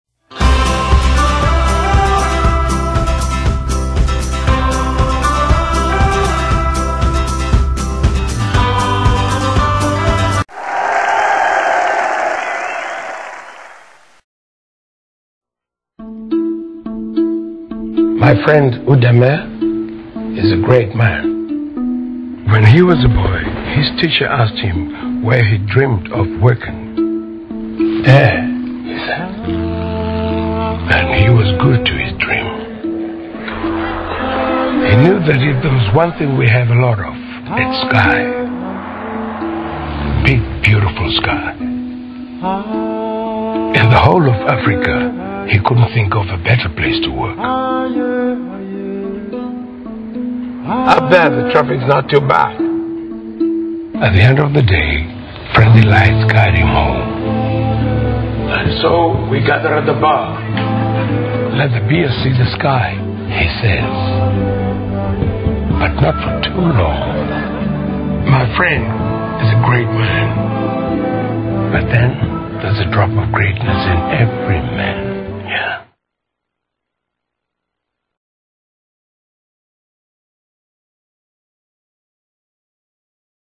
Nigerian TV Ads that are better than movies: No.1 Guiness - Udeme (Reach for Greatness)
Before cable and the internet became mainstream, all most of us had was local television. Perhaps because of our minimal options of television entertainment we actually used sit through all the commercial breaks. Weird, right?

In a compilation of a few favorites, here is no 1 on our list; Guiness' Reach for greatness. Great stuff.